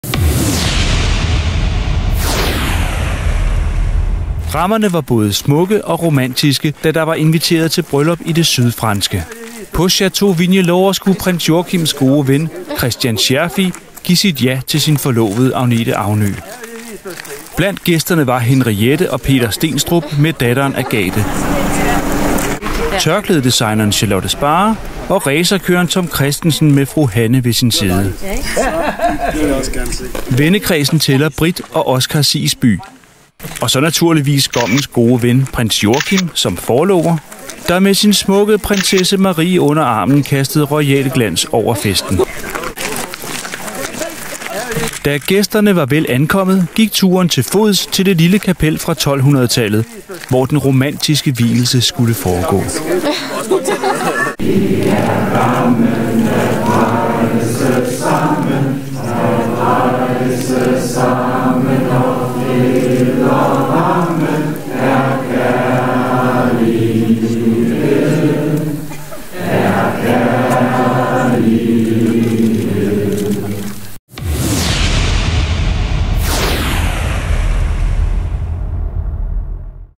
Joachim og Marie til bryllup i Sydfrankrig
Prins Joachim og prinsesse Marie til vennen Christian Scherfigs bryllup i Sydfrankrig.